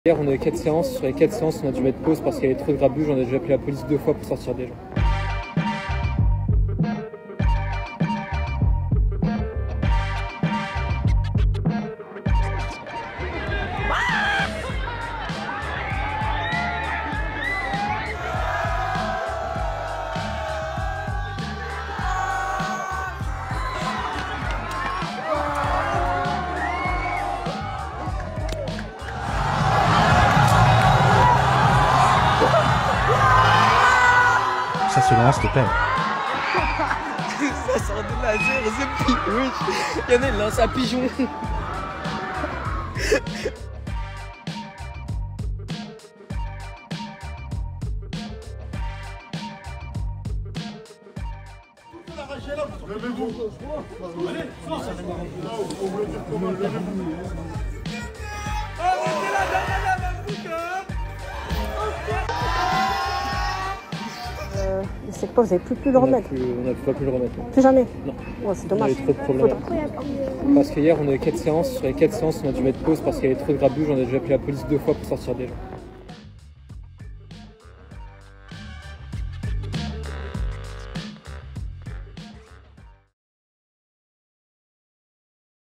Pourquoi "Les Segpa au ski" ne passe plus dans certains cinémas ?
CINÉMA - Victime de son succès. Les Segpa au ski, sorti le 27 décembre, a été déprogrammé de plusieurs cinémas suite à des débordements. Le deuxième volet des aventures des Segpa, réalisé par Hakim et Ali Bougherab et coproduit par Cyril Hanouna, a attiré beaucoup de monde en salles pendant les vacances scolaires, notamment des groupes de jeunes.

Mais de nombreuses séances ont été perturbées par des spectateurs qui criaient et se levaient pendant le film, comme on peut le voir dans la vidéo en tête d’article. Certains ont même été filmés en train de jeter du pop-corn ou encore des chaussures à travers la salle. Au Mégarama de Saint-Étienne, une jeune fille a sauté par-dessus les sièges pour frapper une autre spectatrice, comme l’a rapporté Le Progrès.